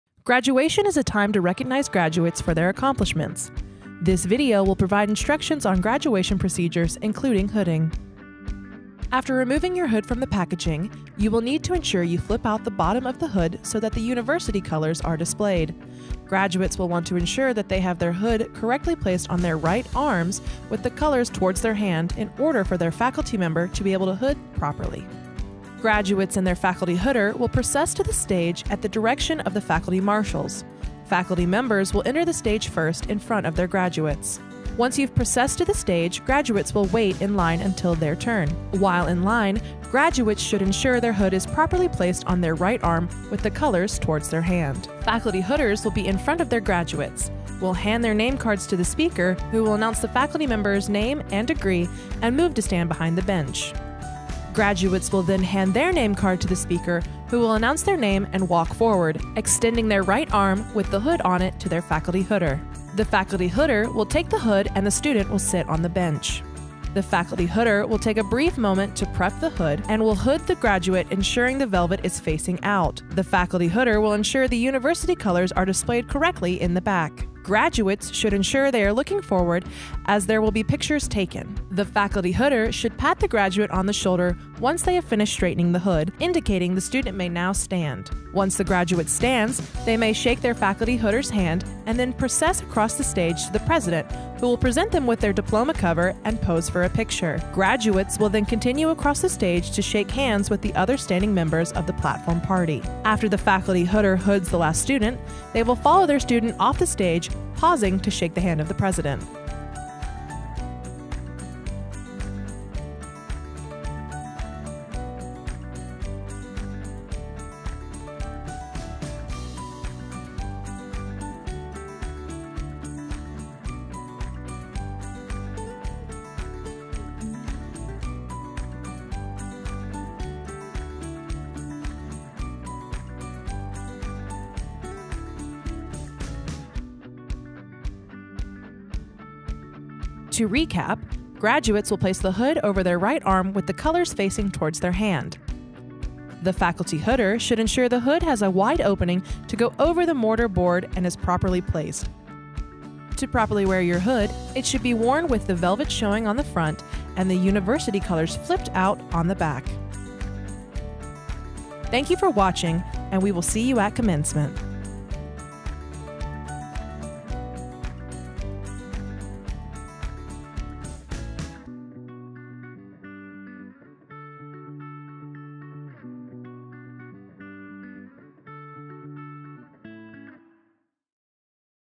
How to Correctly Put on Your Graduation Hood | Valdosta State University
This short but helpful video provides a tutorial explaining how to correctly put on and wear the Master's Hood. Congratulations candidates!